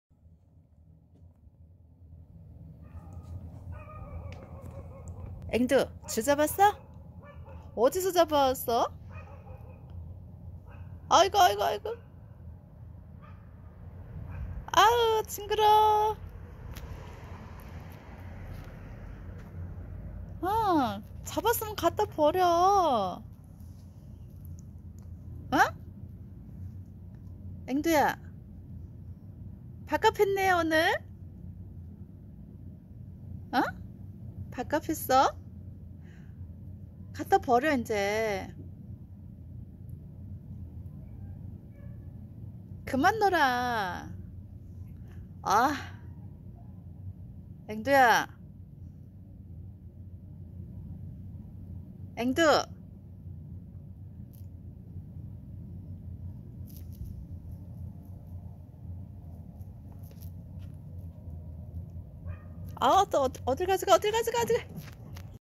길냥이가 밥 값 했어요.ㅋ
아침에 문을 열면 길냥이들이
쥐와 새를 잡아다 놓는 경우가 있어요.
그러면 잘 했다고 칭찬을 해주지요.
길냥이들이 새는 먹고
쥐는 안먹는답니다.